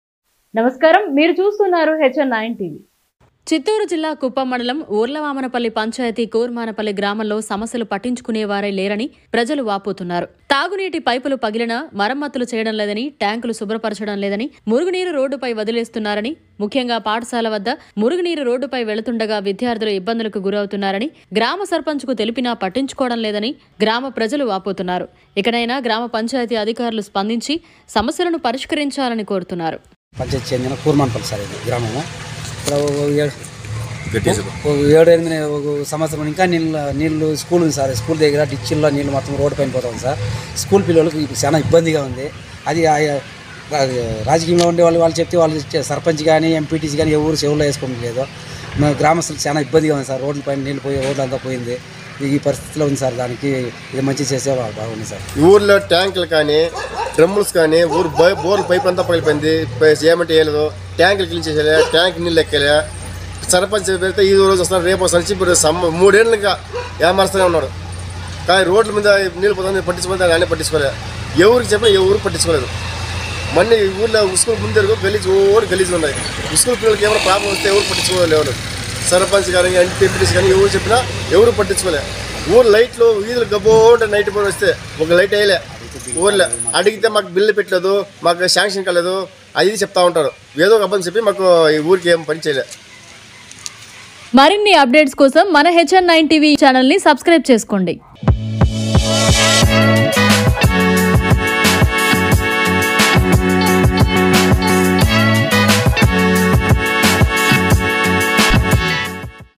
కూర్మానపల్లి గ్రామంలో సమస్యలు పట్టించుకునేవారు లేరు |Kuppam|HN9TV
todaynews newstoday hn9tvnews hn9tv కూర్మానపల్లి గ్రామంలో సమస్యలు పట్టించుకునేవారు లేరు .